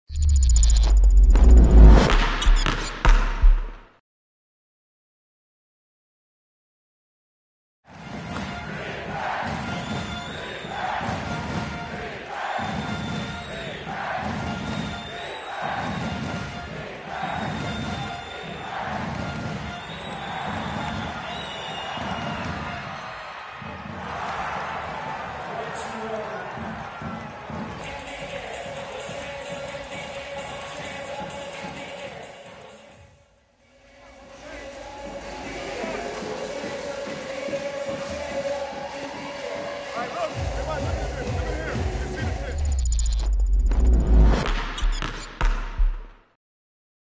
Play of the Game: Reggie Redding (Artland - ALBA HF4)
Reggie Redding mit dem Clutch-Dreier zur Entscheidung für den Finaleinzug (Artland Dragons - ALBA BERLIN, Halbfinale 4, 3.6.2014, Artland Arena).